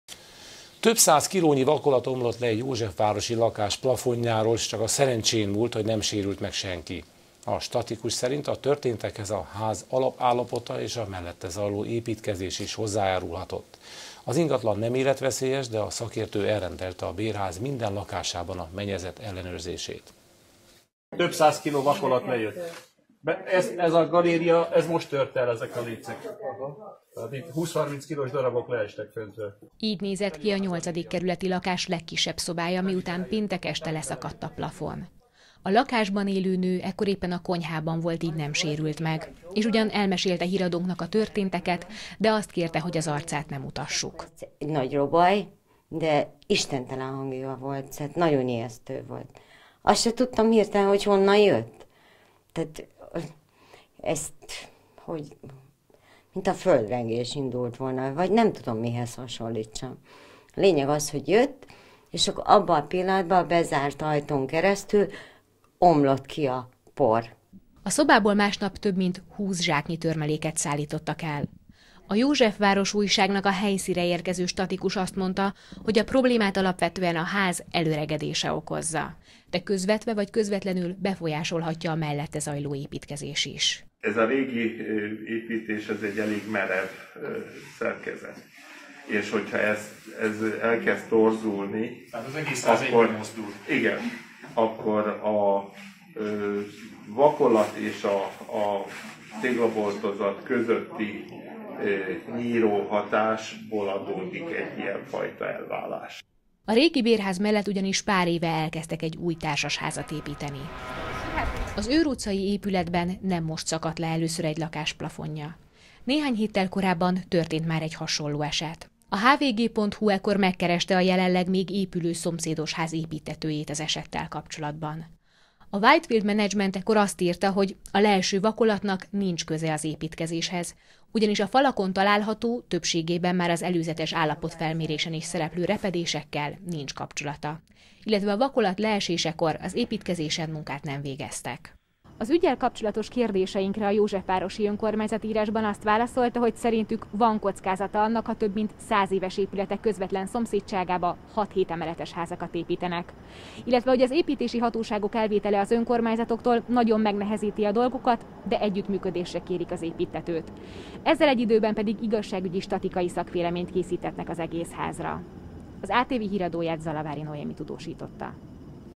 Több száz kilónyi vakolat omlott le egy józsefvárosi lakás plafonjáról
Több száz kilónyi vakolat omlott le egy józsefvárosi lakás plafonjáról. Csak a szerencsén múlt, hogy nem sérült meg senki. A statikus szerint a történtekhez a ház alapállapota és a mellette zajló építkezés is hozzájárulhatott. Az ingatlan nem életveszélyes, de a szakértő elrendelte a bérház minden lakásában a mennyezet ellenőrzését.